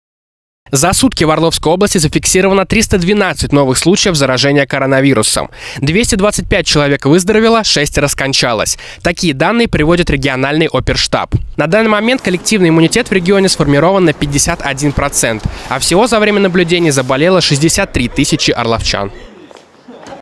В Орловской области снижаются темпы заболеваемости коронавирусом
За сутки в Орловской области зафиксировано 312 новых случаев заражения коронавирусом. 225 человек выздоровело. Такие данные приводит региональный оперштаб. Для сравнения, в период нерабочей недели в стуки на территории Орловской области ежесуточно фиксировалось порядка 350 новых случаев заболевания.

 На данный момент коллективный иммунитет в регионе сформирован на 51 процент. А всего за время наблюдений заболело 63 тысячи человек.